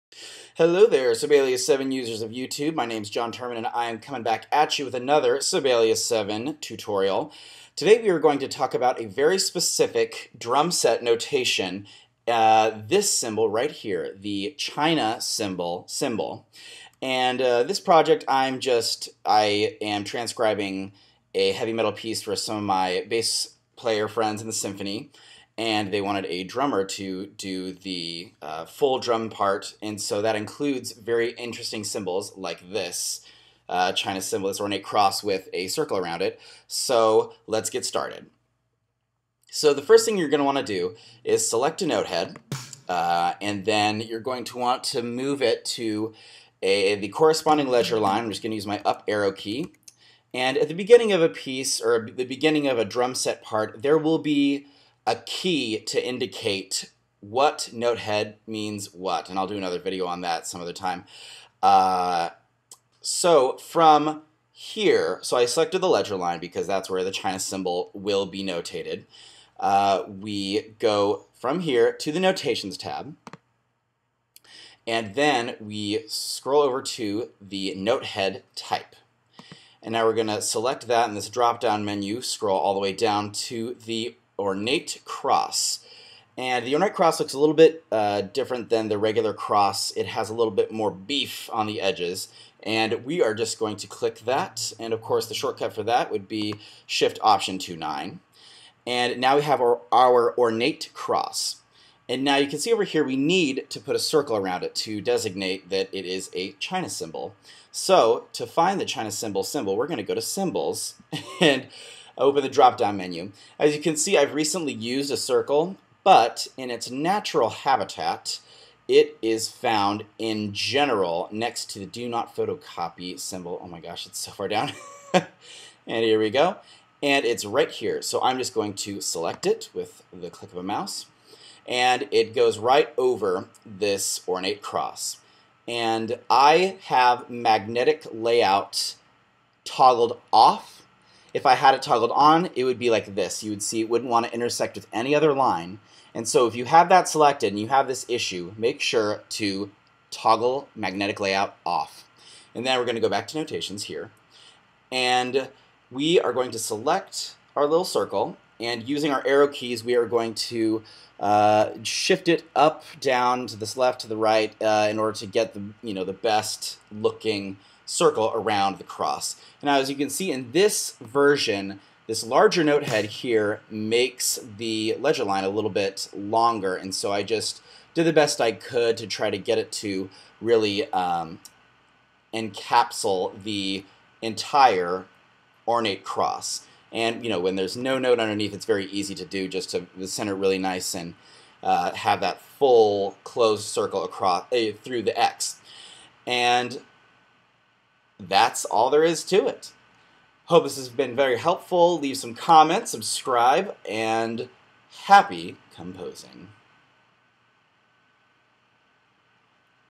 NOTATE "CHINA CYMBAL" | DRUMSET NOTATION | Sibelius Tutorial | Composer's Guide
This tutorial covers the process of changing a regular notehead to an ornate cross with a circle. This is usually used to designate a "China Cymbal" hit in a drum set part.

This hack works for versions of Sibelius 7 and higher.